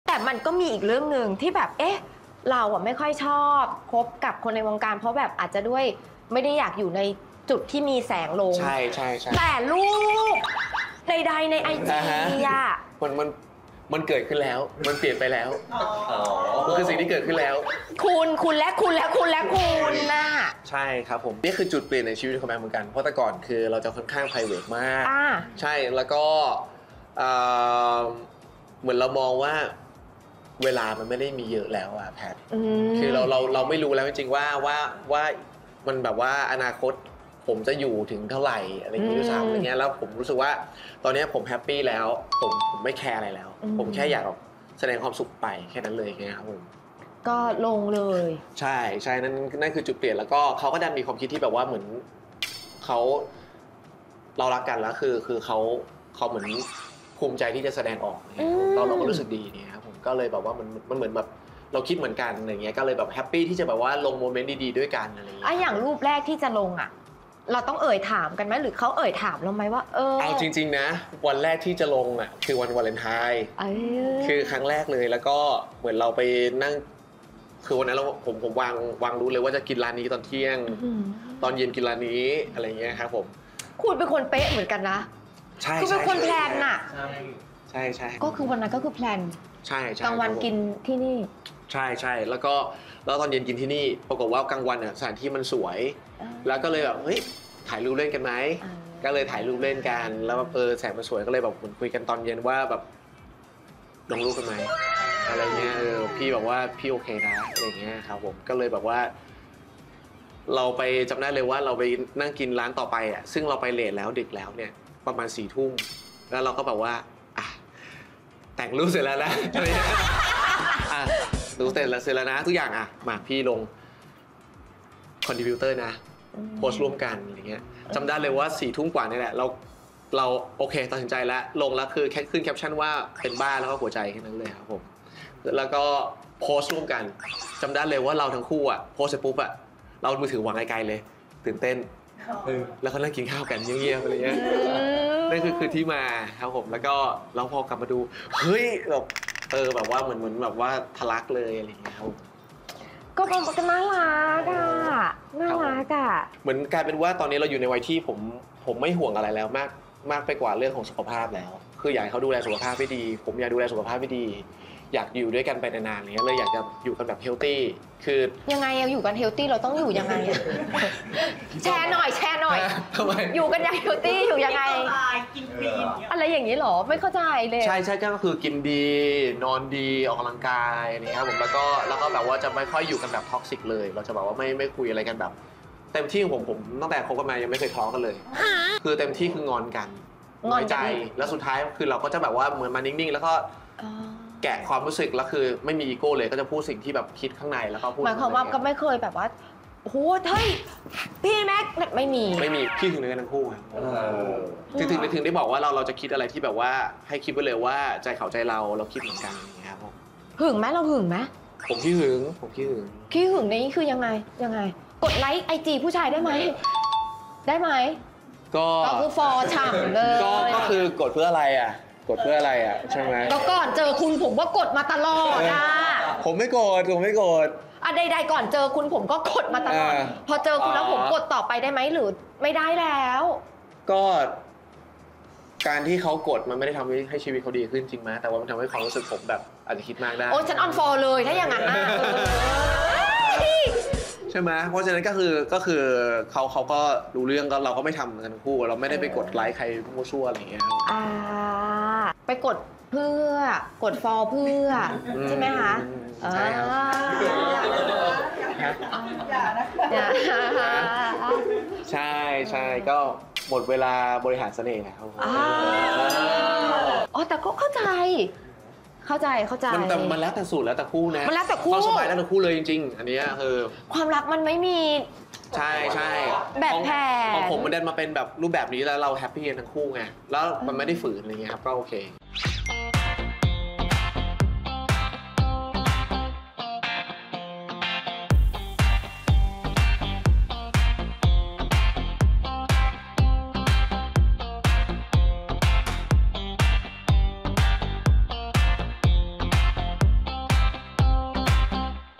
"แม็ค วีรคณิศร์" เผยคนนี้เป็นจุดเปลี่ยนของชีวิต ขี้หึงทั้งคู่ หมดเวลาบริหารเสน่ห์ | โดนเทเซมาที่แพท
"แม็ค วีรคณิศร์" เผยคนนี้เป็นจุดเปลี่ยนของชีวิต รับขี้หึงด้วยกันทั้งคู่ หมดเวลาบริหารเสน่ห์ ตอนนี้ชีวิตแฮปปี้ ตั้งแต่คบกันมาไม่เคยทะเลาะกันเลย